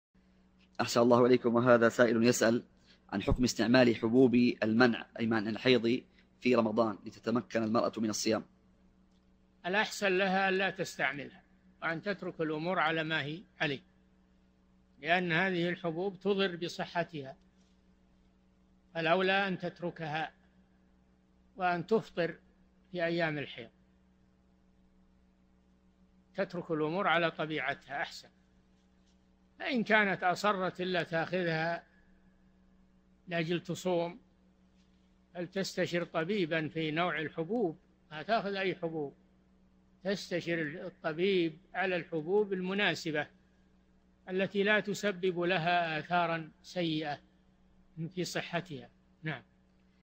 [145 /186] يسأل عن حكم استعمال حبوب منع الحيض في رمضان لتتمكن المرأة من الصيام؟ | الشيخ صالح الفوزان
[145 /186] يسأل عن حكم استعمال حبوب المنع أي منع الحيض في رمضان لتتمكن المرأة من الصيام؟
 الجواب: الأحسن لها ألا تستعملها وأن تترك الأمور على ما هي عليه؛ لأن هذه الحبوب تضر بصحتها فالأولى أن تتركها وأن تفطر في أيام الحيض، تترك الأمور على طبيعتها أحسن فإن كانت أصرت إلا تأخذها لأجل تصوم فلتستشر طبيبًا في نوع الحبوب ما تأخذ أي حبوب تستشر الطبيب على الحبوب المناسبة التي لا تسبب لها آثار سيئة في صحتها.
السيرة الذاتية :
للاشتراك في قناة الشيخ صالح الفوزان

للاستفسار والإقتراحات والدعم
رئيس الفريق
لمشروع كبار العلماء